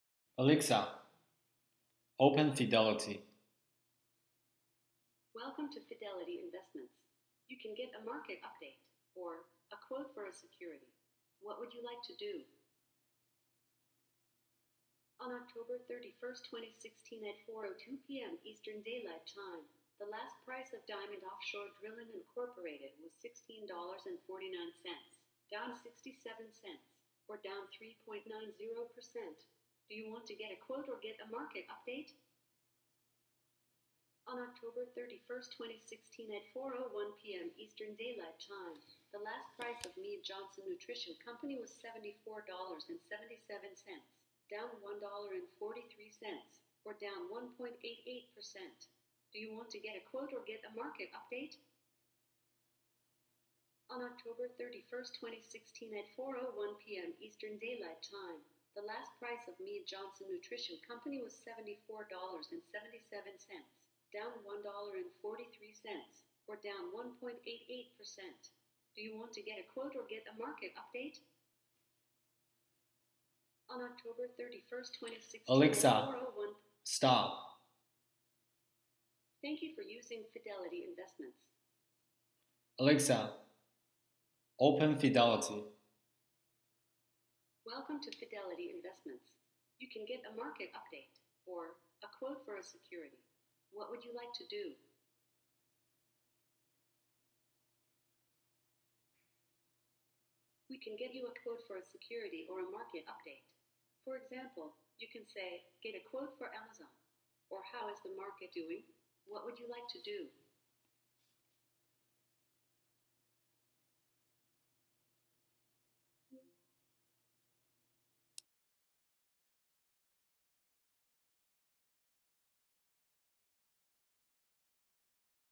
Echo Dot 2nd Gen Review. Problem with microphone arrays?
I saw many people complaining about the dumbness of their Echo Dot, so I recorded this video trying to figure out if the problem is from the microphone system by comparing testing same skill when the microphone is on and muted. Even no command is given, Echo Dot still thinks it heard something. Any thoughts?